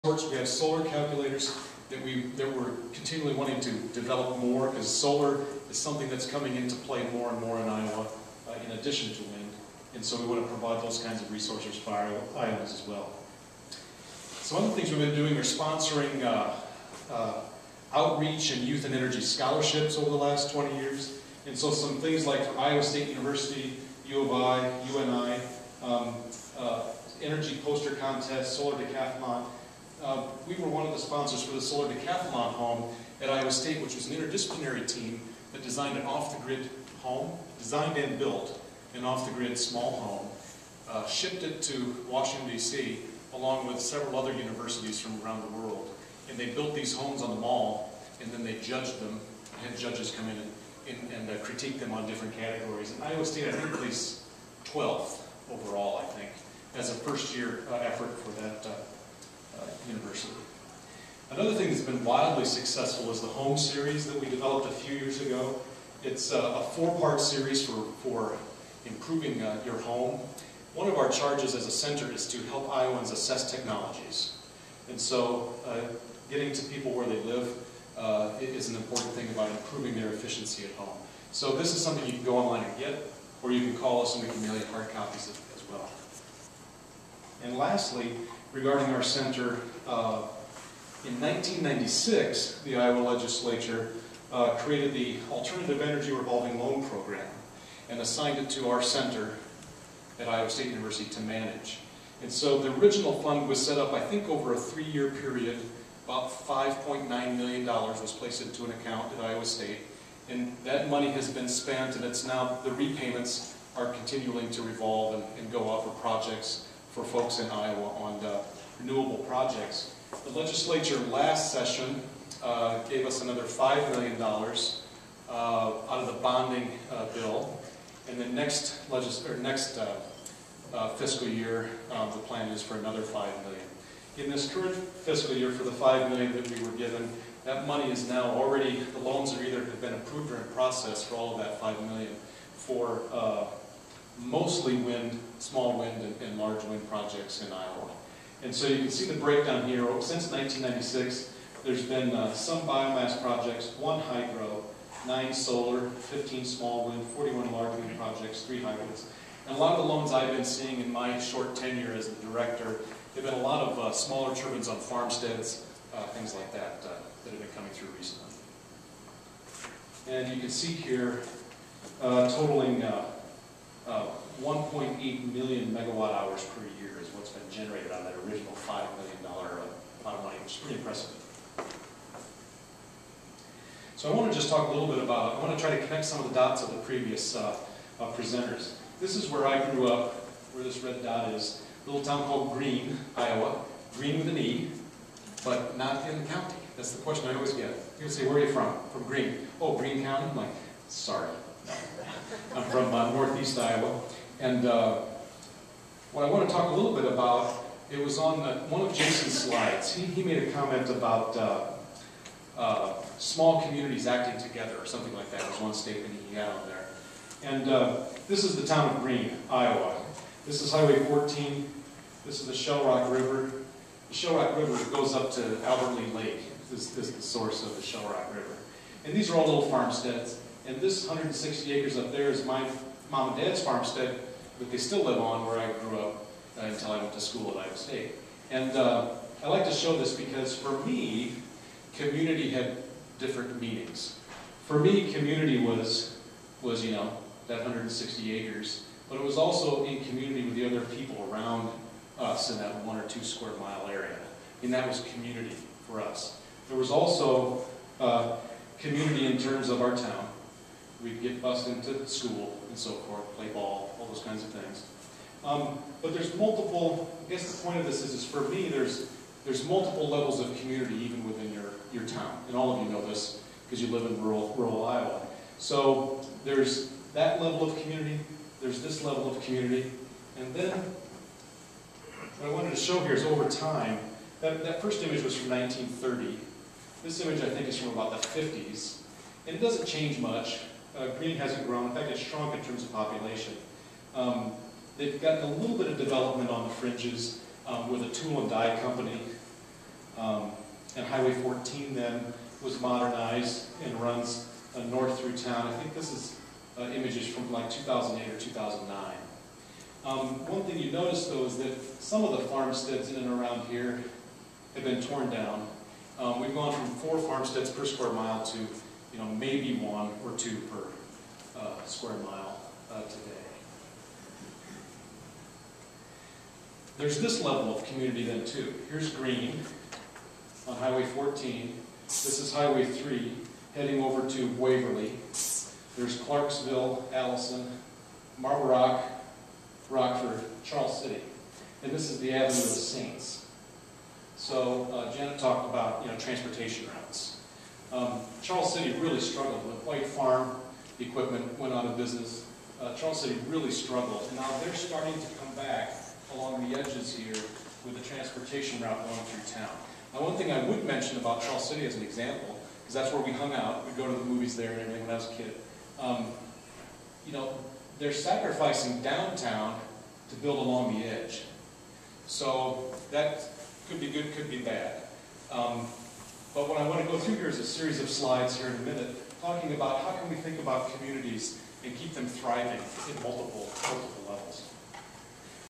MWP 2010 Annual Dinner - Nordmeyer 2
The Midwest Partnership Development Corporation held its annual dinner on January 21.  The keynote speaker was Kevin Nordmeyer, Director of the Iowa Energy Center.  Midwest Partnership supports economic development for four counties in west central Iowa.